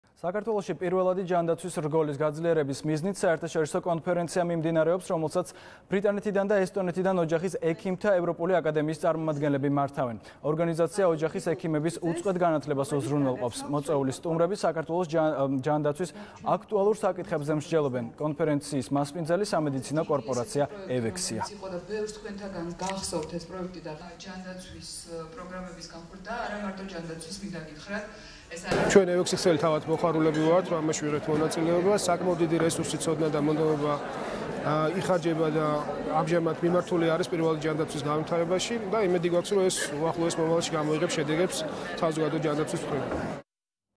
პირველადი ჯანდაცვა - საერთაშორისო კონფერენცია თბილისში - მაესტრო
16 აპრილს, პირველადი ჯანდაცვის განვითარების თემაზე საერთაშორისო დონის კონფერენცია გაიმართა.
ეს ღონისძიება ხელს შეუწყობს პირველადი ჯანდაცვის რგოლის გამართულ და მაღალკვალიფიციურ მუშაობას. კონფერენცია სასტუმრო ”რედისონში” გაიმართა.
მოწვეულ სტუმრებს შორის იყვნენ ოჯახის ექიმთა ევროპული აკადემიის (EURACT) წარმომადგენლები ბრიტანეთიდან და სლოვენიიდან.